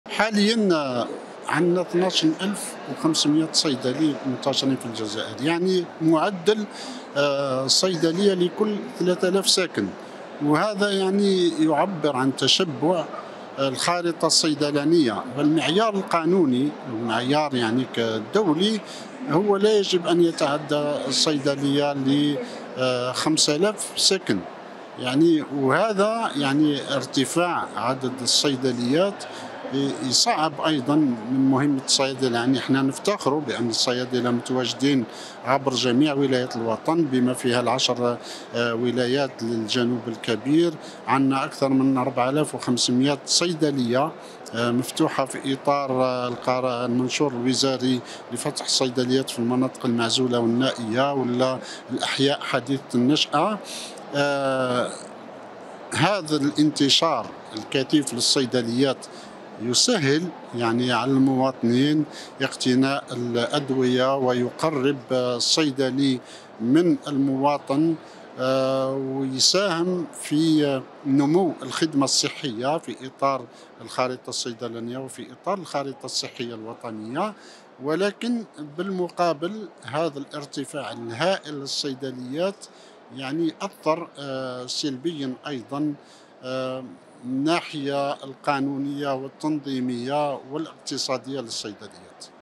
بالأرقام نقابة الصيادلة الخواص تكشف:
⭕️12500صيدلية في الجزائر بمعدل صيدلية لكل 3000ساكن
⭕️المعيار القانوني صيدلي لكل 5000 ساكن
⭕️4500صيدلية في المناطق المعزولة